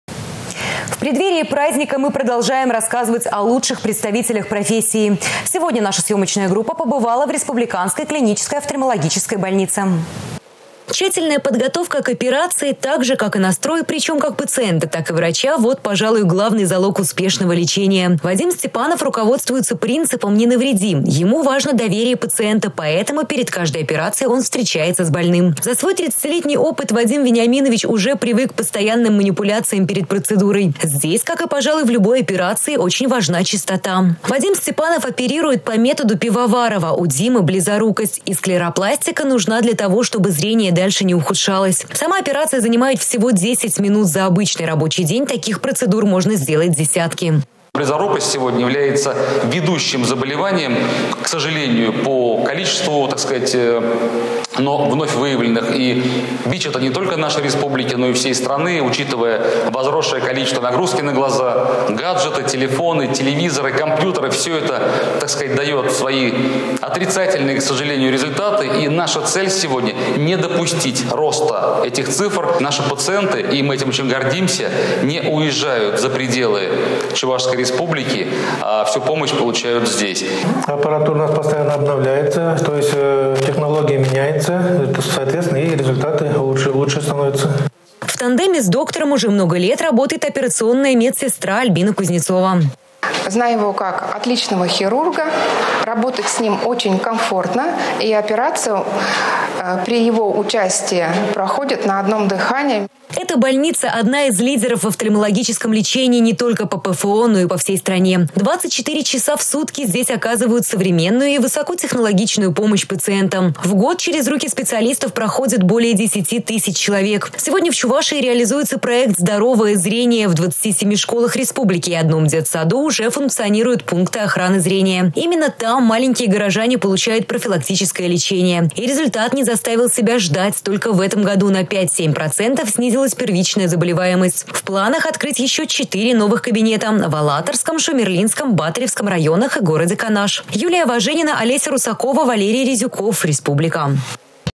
Лучший офтальмолог в Чувашии
В преддверии праздника мы продолжаем рассказывать о лучших представителях профессии. Сегодня наша съемочная группа побывала в республиканской клинической офтальмологической больнице.